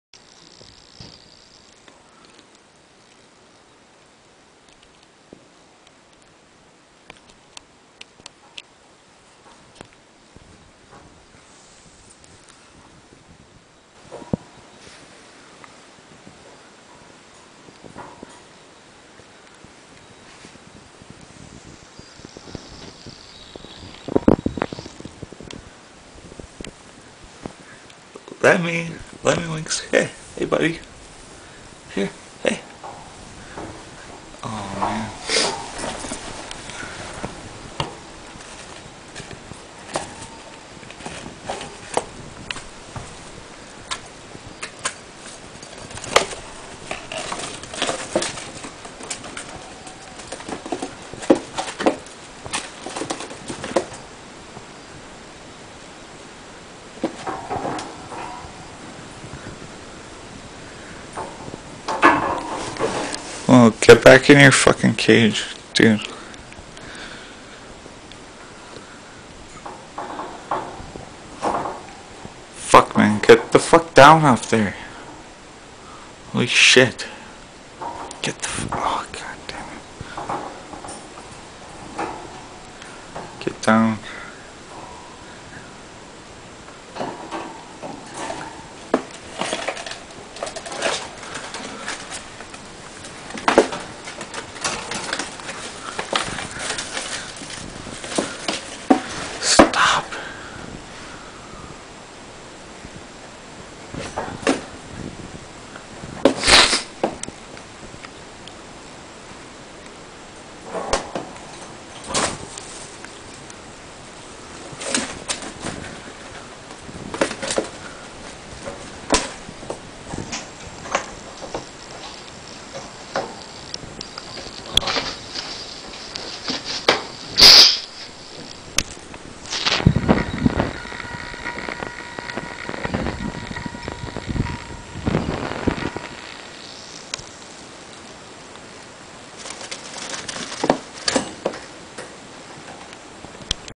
Sick rat loss of balance
my rat lemiwinks has gotten i'll very rapidly over the past few days.  My rat is a female and is about a year old.  almost everyday i will open my rats cage and leave it open so she can freely come out and explore a bit.  I could leave her unattended for a couple hours n she would always stay near or in her cage.  About 4-5 days ago i noticed she was having a hard time climbing out of the cage, kinda stumbling n missing the rungs of the cage, sometimes falling off.  I also noticed she was walking with her head tilted slightly.  she also had red stuff on her forearms, which i thought usually came from the eyes... perhaps she rubbed her eyes or something.. over the next couple days her climbing got worse very quickly to the point where she cannot climb without falling.  yesterday when i brought her veggies she stood up on her back feet to grab the food and falls over.  made me very sad to see her in this condition and i had to move her cage out of my bedroom and into another room so i wasnt watching it suffer..  when i came home from work today to check on her i tapped on her cage which usually makes her come out of her house but she wouldnt come out.. so i banged on her house and picked up the house and shook it and she didnt move i thought she was dead n left the room... then came back a few minutes later and she was out walking around very unbalanced stumbling around everywhere like she is drunk.